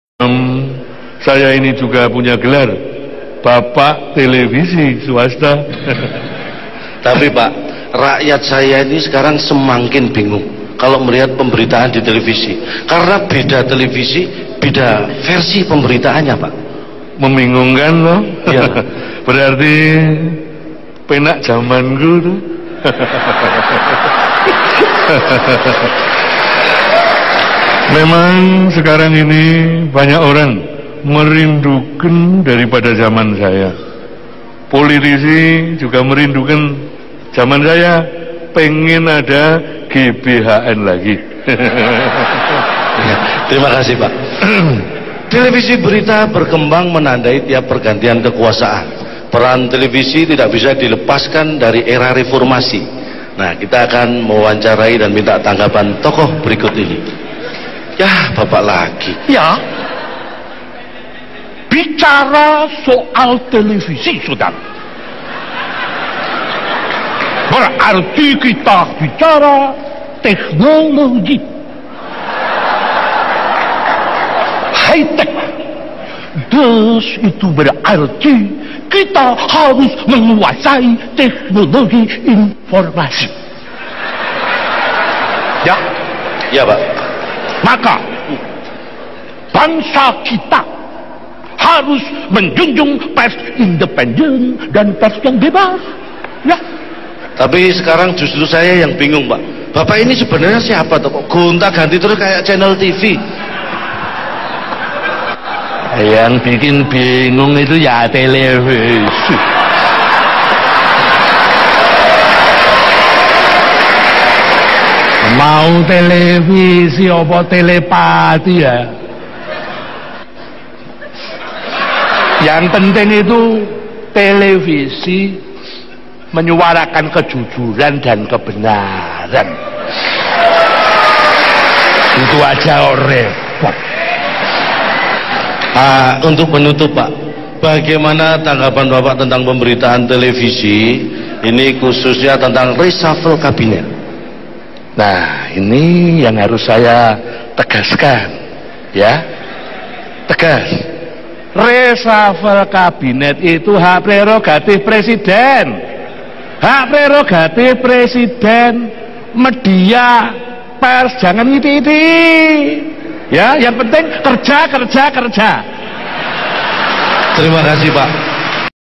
Presiden Presiden RI (Lawakan Butet Kartanegara ft. Cak Lontong)
Tampil bersama Cak Lontong, Budayawan Butet Kartanegara menirukan gaya bicara beberapa Presiden RI, antara lain :
- Presiden Soeharto
- Presiden BJ. Habibie
- Presiden Abdurrahman Wahid
- Presiden Megawati Soekarnoputri

Adapun para pejabat yang menonton acara tersebut ketawa terpingkal pingkal, antara lain :
- Jusuf Kalla
- Badrodin Haiti
- Ferry M. Baldan
- dll.